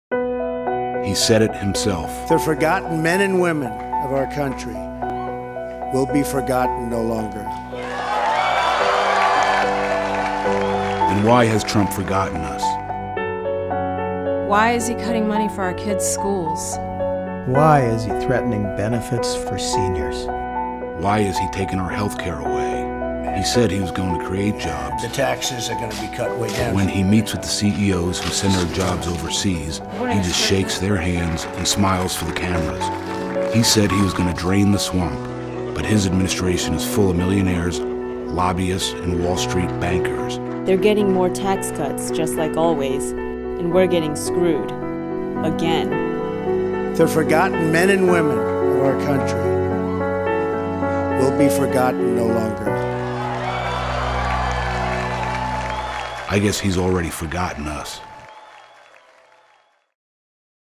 Forgotten | Explore the Issues | AFSCME Video
Donald Trump ran on a promise to lift up the forgotten man and woman, but he has already forgotten us. He puts corporations and the wealthy first. Now he’s turning his back on those who elected him again by laying out a budget for 2018 that lowers living standards for working and middle-class Americans, all to give tax cuts to the wealthy and privileged few.